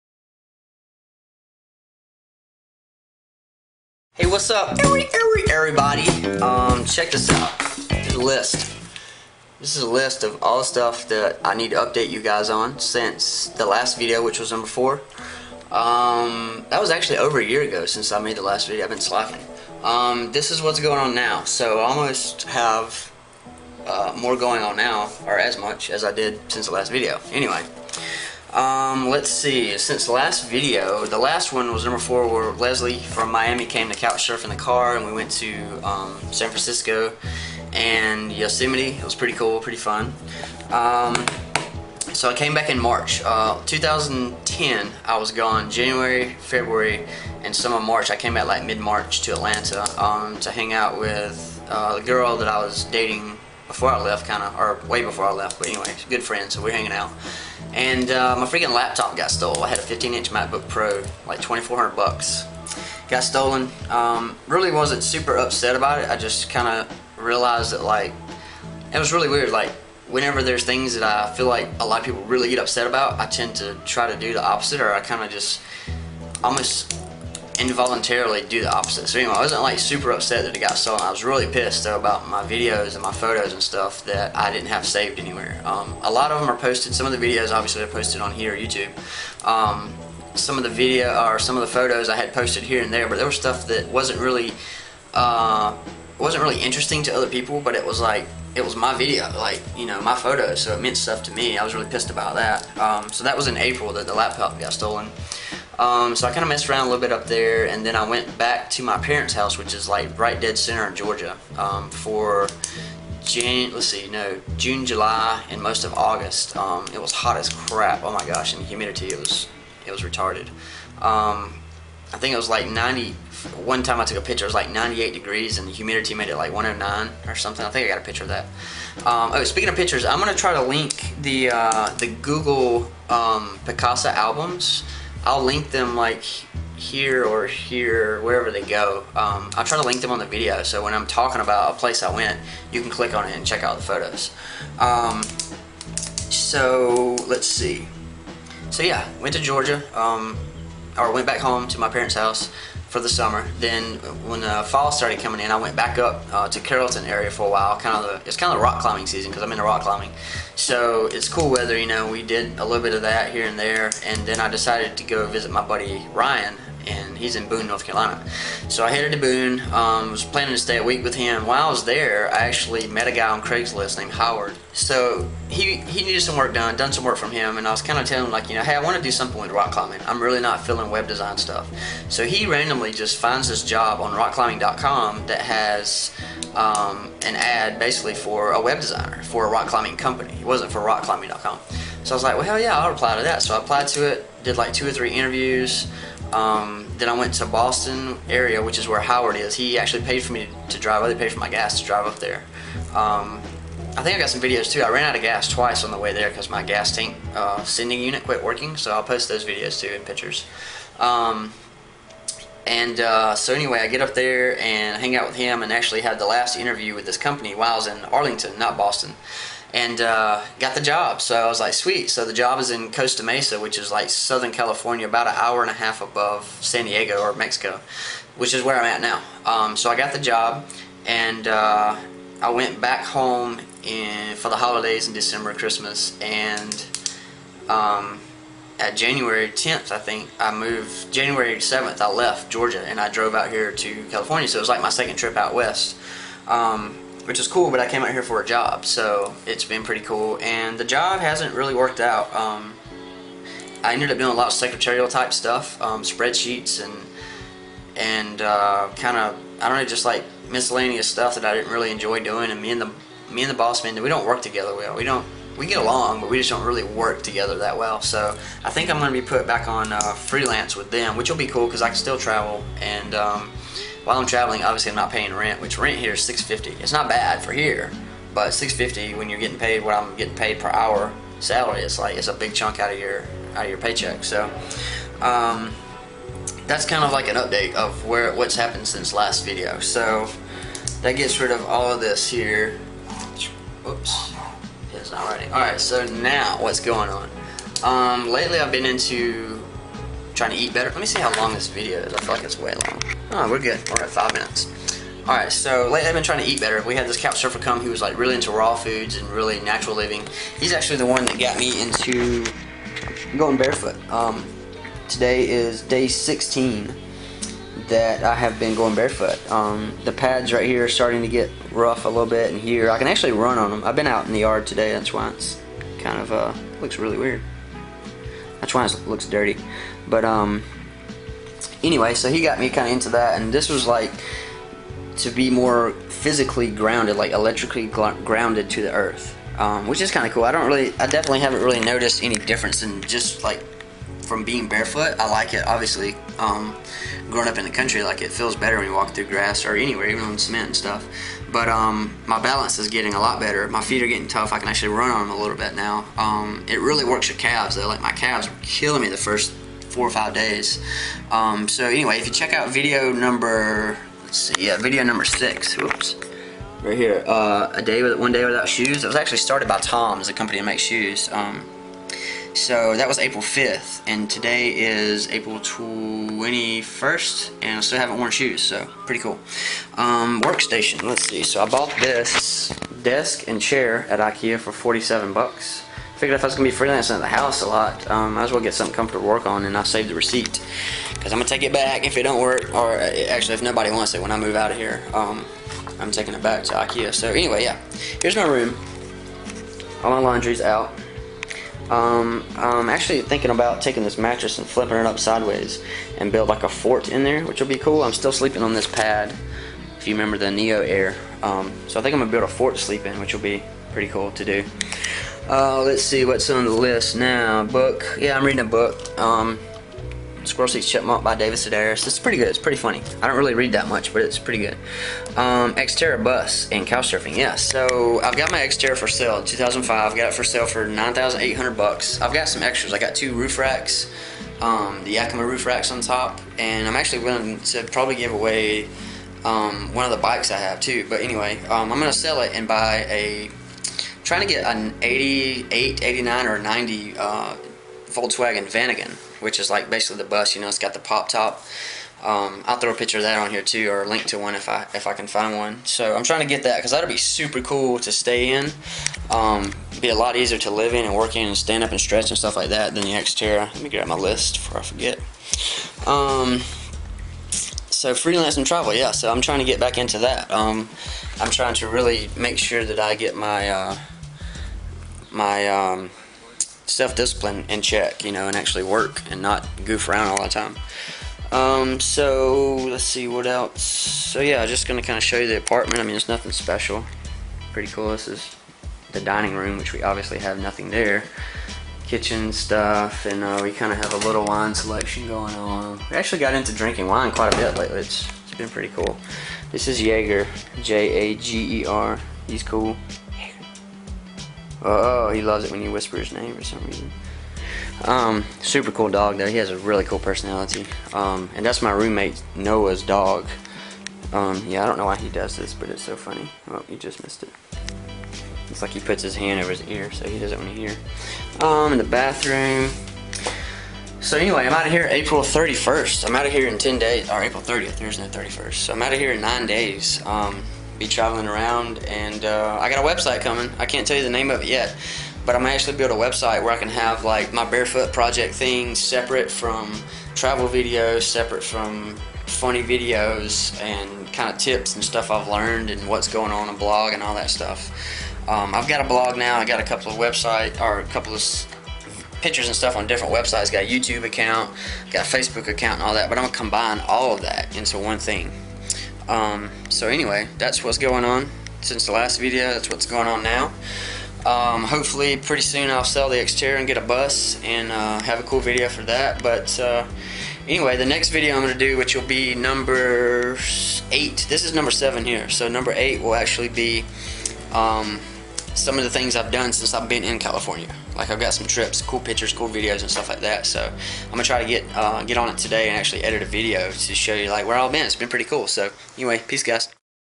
Going Barefoot for 41 Days - These Are The First 16
Moved to Costa Mesa to start a new job. An April 5th, the shoe company TOMS encouraged people to go to work barefoot in order to raise awareness that not everyone has a pair of shoes. I decided to see just how long I could go barefoot without being asked to put shoes on. Ended up being asked at a bar in Seal Beach, CA after 41 days.